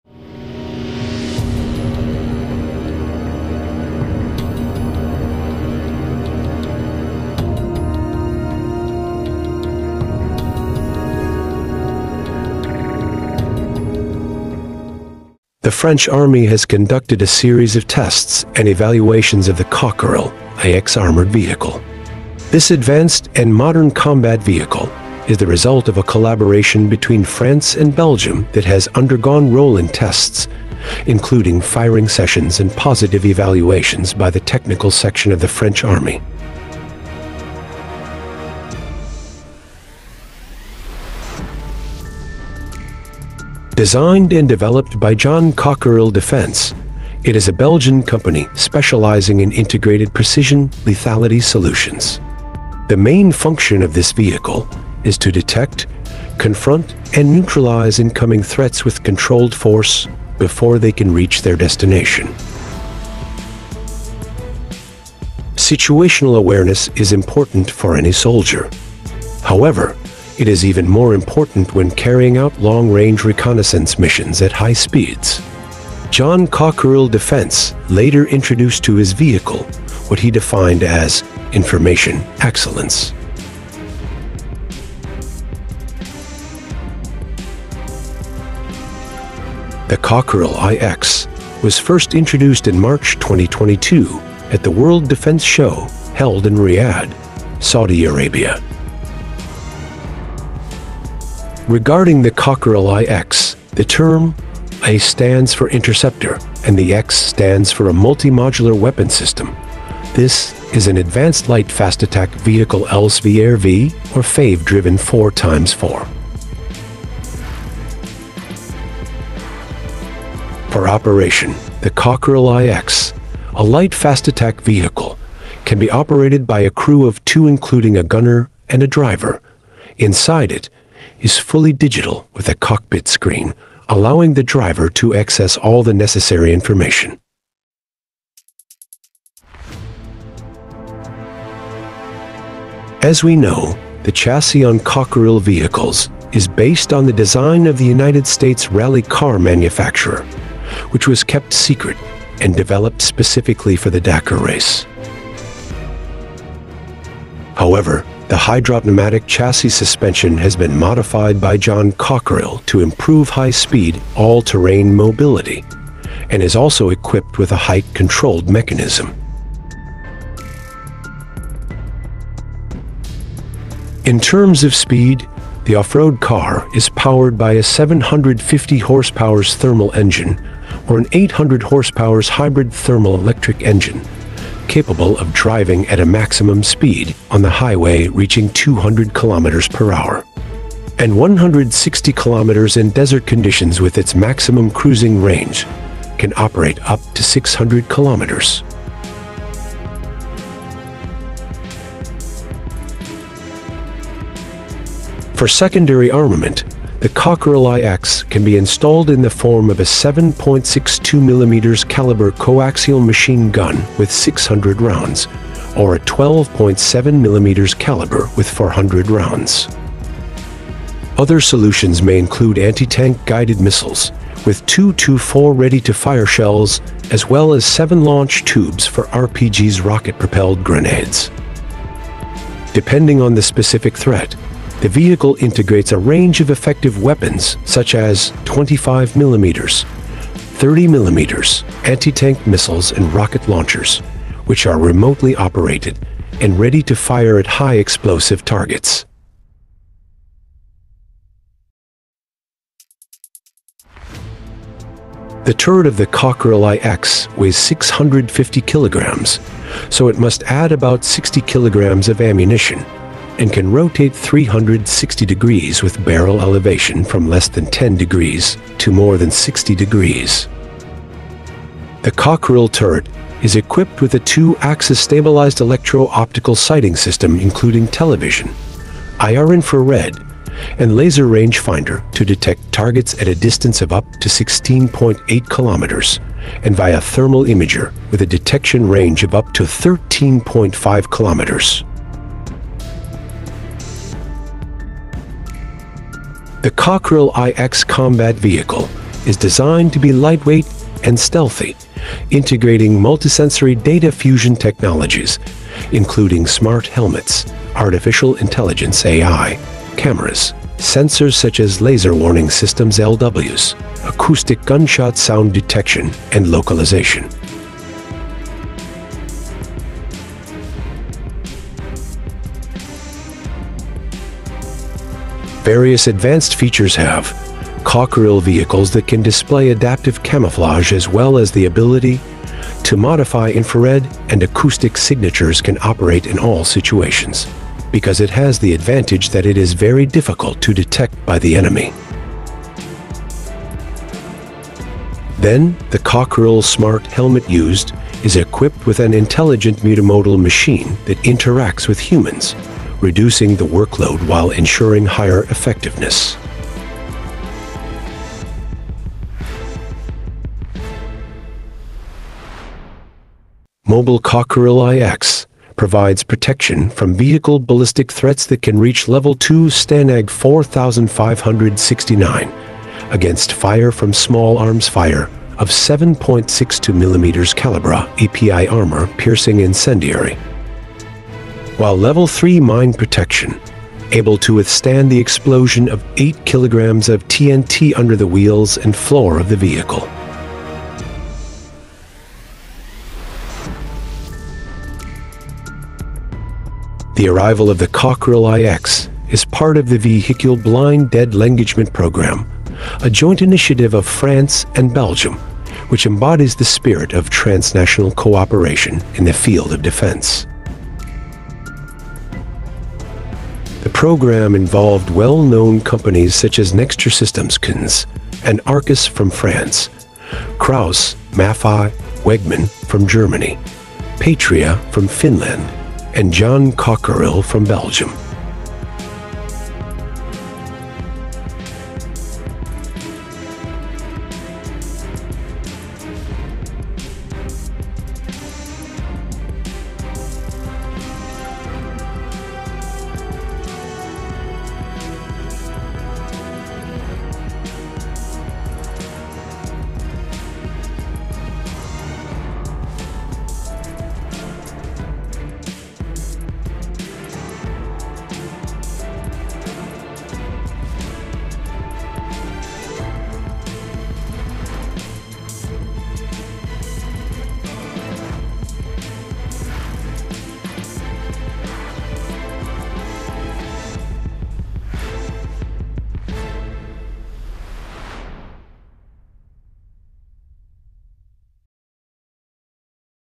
Terrible!! French Army Launches Combat Vehicle With The Deadliest Weapon - Cockerill i-X
The French Army has conducted a series of tests, and evaluations of the Cockerill® i-X armored vehicle.

The Cockerill i-X is designed as a lightweight and stealthy vehicle, integrating multisensory data fusion technologies, including Smart Helmets, Artificial Intelligence (AI), cameras, sensors such as Laser Warning Systems (LWS), Acoustic Gunshot Sound detection and localization. The vehicle integrates a range of effective weapons such as 25mm, 30mm caliber cannons, anti-tank missiles and rocket launchers, which are remotely operated and ready to fire at high-explosive targets.